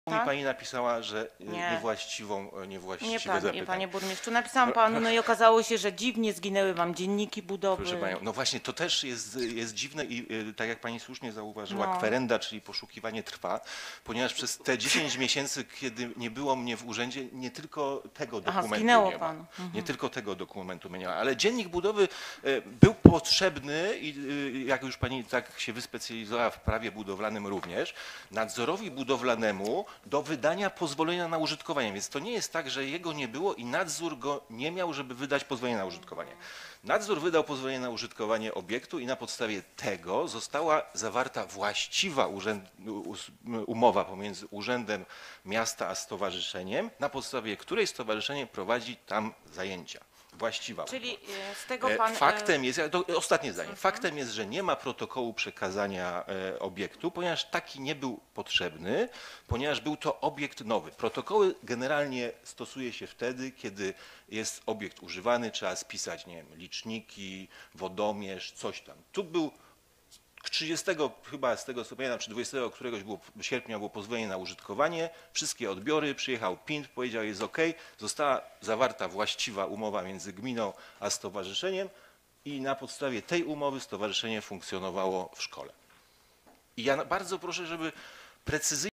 Zagniniony dziennik budowy szkoły w Niepołomicach-Jazy
Fragment sesji Rady Miejskiej w Niepolomicach z dnia 23 listopada 2021. Radna prosi o udostępnienie dziennika budowy szkoły, jednak jest problem z jego odnalezieniem.